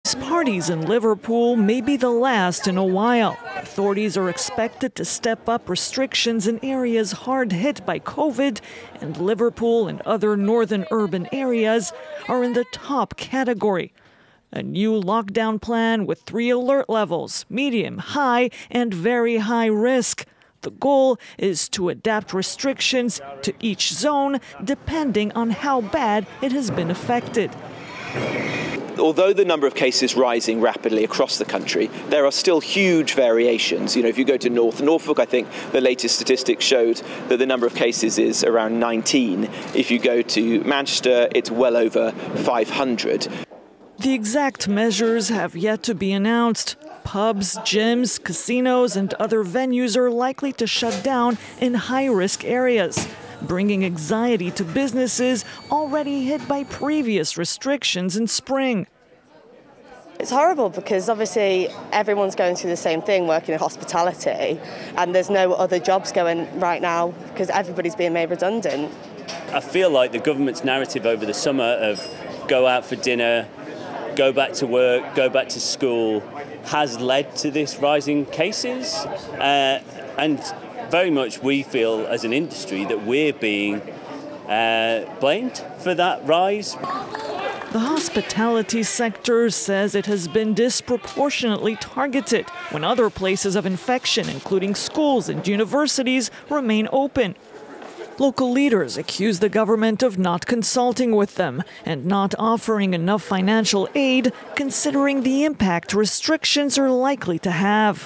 England new lockdown rules: Liverpool to be placed under tight restrictions
The city of Liverpool will go into the strictest "third tier" of new anti-coronavirus restrictions to be announced imminently by Britain, its leaders said late on Sunday after talks with the British government. The government has decided that further measures and closures will apply to Liverpool, the city's leaders, including Mayor Steve Rotheram, said in a joint statement.